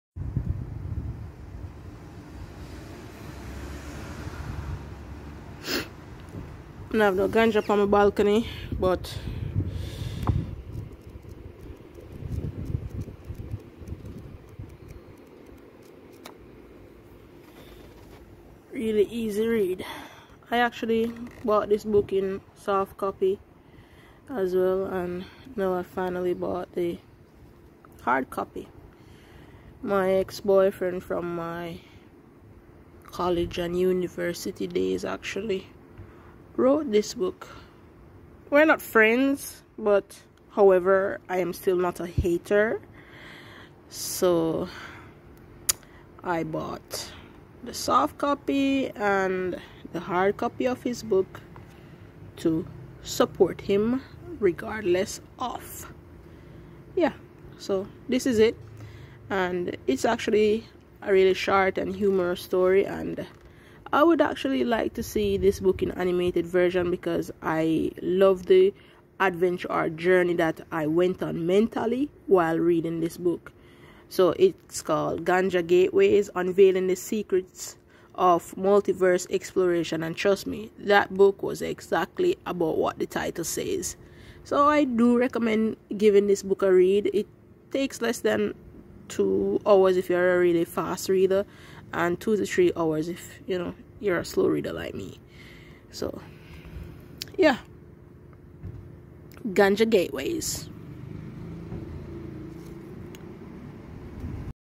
Finally got a hard copy of the book that my ex wrote and I absolutely love the cover with the glossy
Ganja Gateways: Unveiling the secrets of Multiverse exploration